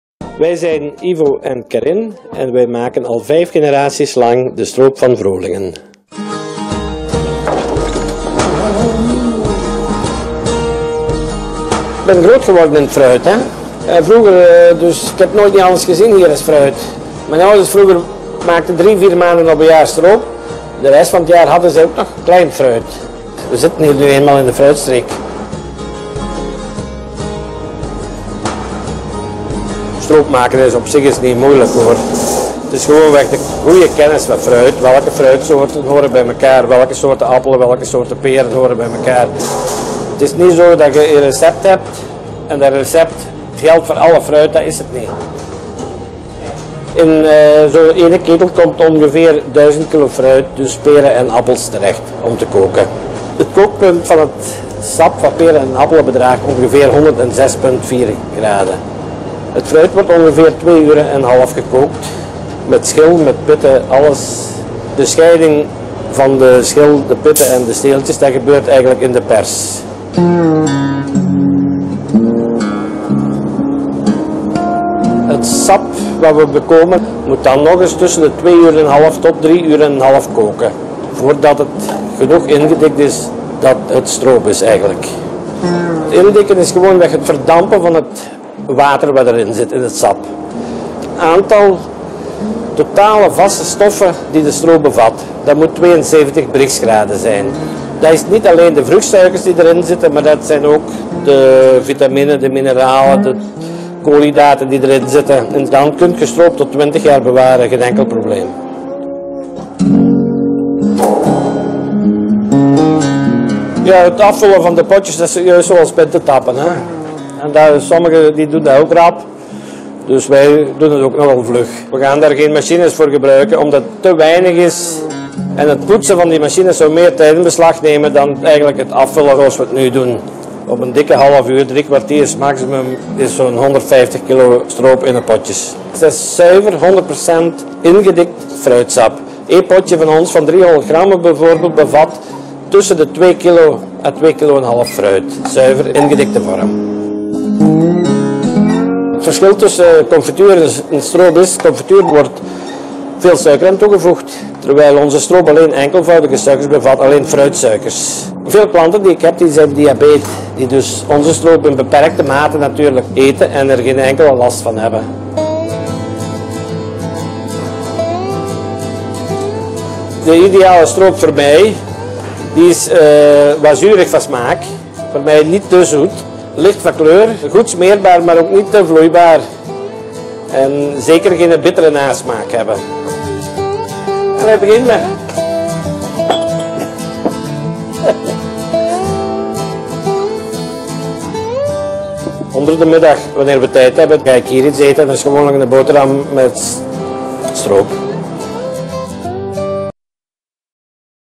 Stroop van vrolingen
Sinds 1843 wordt er in Vrolingen op ambachtelijke wijze stroop gestookt. Hij bevat geen bewaarmiddelen, kleurstoffen, extra suikers of smaakstoffen. De appels en peren worden zorgvuldig geselecteerd, gekookt en uitgeperst. Deze bereidingswijze geeft aan de stroop zijn onovertroffen smaak en uitzicht. In de stroopstokerij kan je ook terecht om je eigen fruit te laten verwerken.